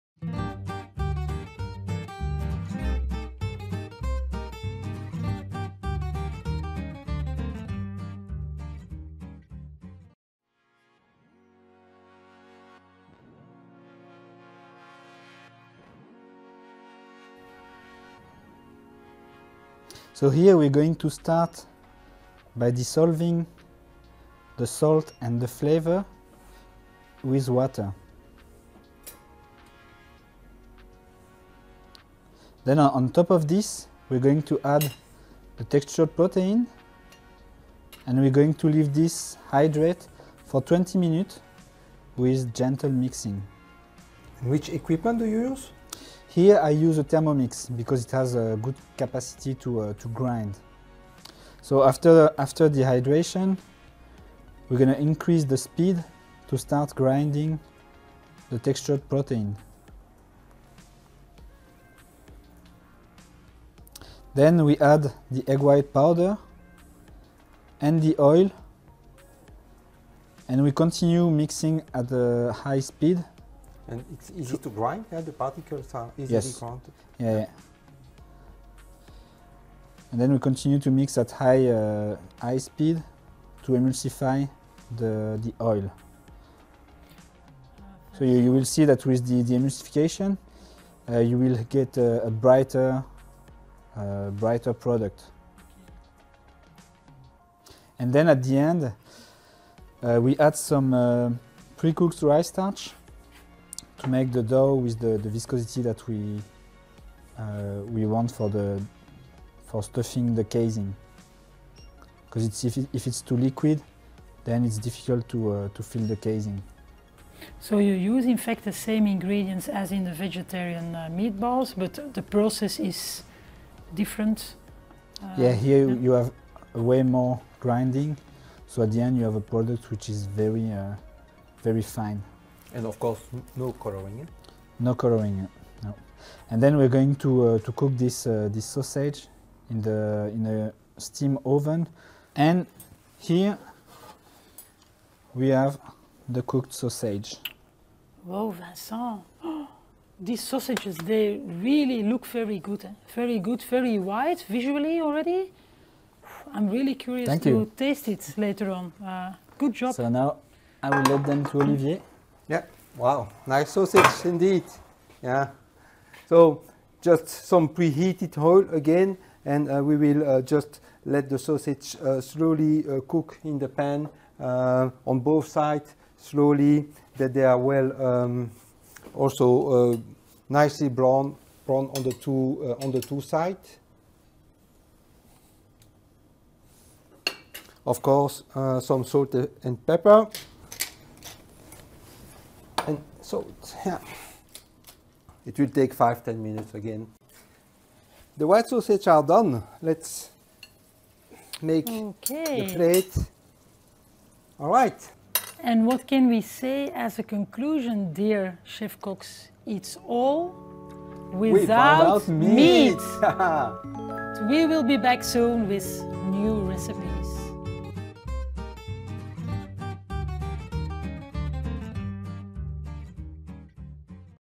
Vegetarian White Sausages with Textured Wheat Protein
Find out how to prepare delicious vegetarian white sausages with  BeneoPro W-Tex.